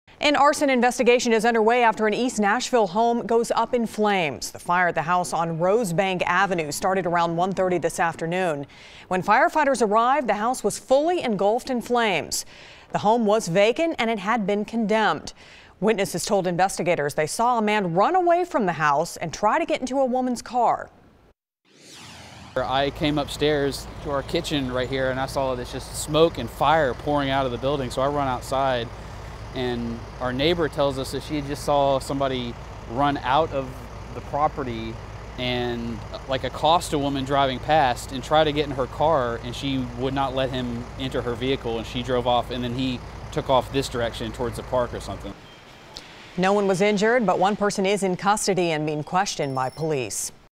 1 Arrested After Fire On Rosebank Avenue
One man has been arrested after a fire on Rosebank Avenue. The fire happened in the 800 block at a vacant home that has now been condemned.